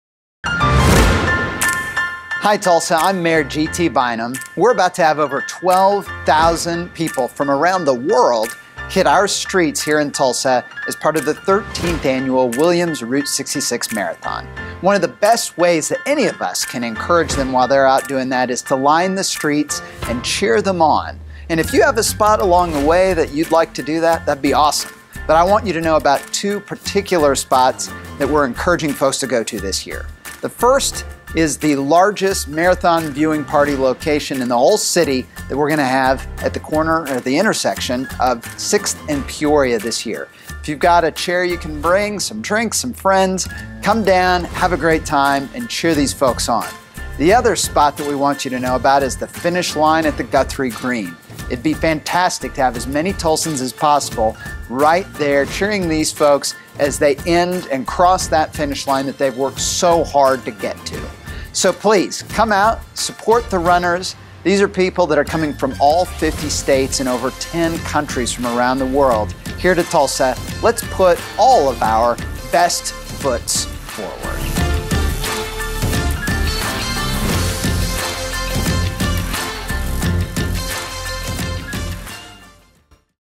Mayor G.T Bynum invites Tulsans to A Marathon-Sized Party!
Hey Tulsa! it's time to party. We are about to have 12,000 runners in downtown Tulsa to lace up their sneakers and hit the pavement for the 13th annual Williams Route 66 Marathon presented byBlue Cross and Blue Shield of Oklahoma

If you or a friend live along the course, you can host your own block party. Maybe you eat brunch in your front yard or ask the neighborhood kids to make signs. All we ask is that you cheer on our runners and show them Tulsa at its best.

This year, a new party is set to rock the route. 6th & Peoria will be the new home to Tulsa's biggest marathon party. Bring a lawn chair, some drinks and get ready to cheer!

Or head out to the finish line at Guthrie GreenGreen and enjoy live music while runners crush their goals and cross the finish line.

or on social @Route66marathon